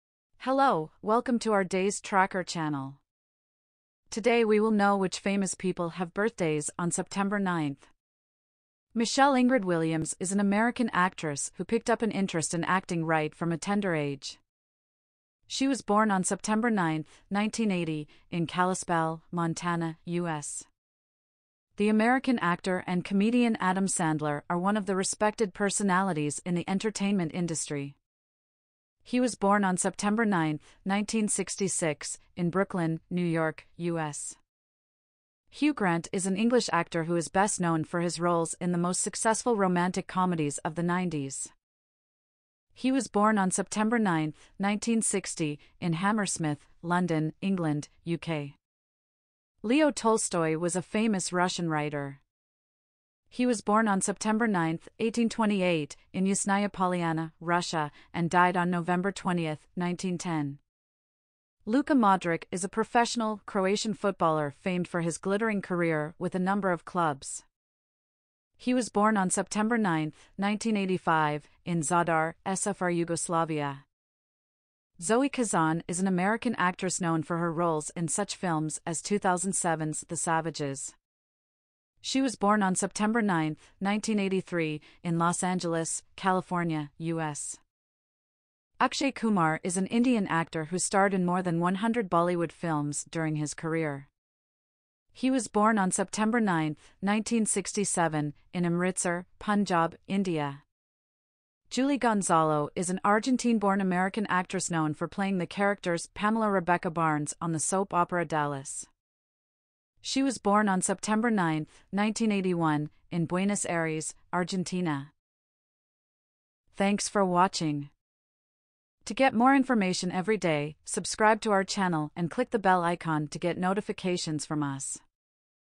September 9: Famous People Born Today
Hello, welcome to our Days Tracker channel. Today we will know which famous people have birthdays on September 9

Michelle Ingrid Williams is an American actress who picked up an interest in acting right from a tender age.
She was born on September 9, 1980, in Kalispell, Montana, U.S.
The American actor and comedian Adam Sandler are one of the respected personalities in the entertainment industry.
He was born on September 9, 1966, in Brooklyn, New York, U.S.
Hugh Grant is an English actor who is best known for his roles in the most successful romantic comedies of the 90s
He was born on September 9, 1960, in Hammersmith, London, England, UK.
Leo Tolstoy was a famous Russian writer.
He was born on September 9, 1828, in Yasnaya Polyana, Russia, and died on November 20, 1910
Luka Modrić is a professional Croatian footballer famed for his glittering career with a number of clubs.
He was born on September 9, 1985, in Zadar, SFR Yugoslavia
Zoe Kazan is an American actress known for her roles in such films as 2007's The Savages,
She was born on September 9, 1983, in Los Angeles, California, U.S.
Akshay Kumar is an Indian actor who starred in more than 100 Bollywood films during his career.
He was born on September 9, 1967, in Amritsar, Punjab, India
Julie Gonzalo is an Argentine-born American actress known for playing the characters Pamela Rebecca Barnes on the soap opera Dallas
She was born on September 9, 1981, in Buenos Aires, Argentina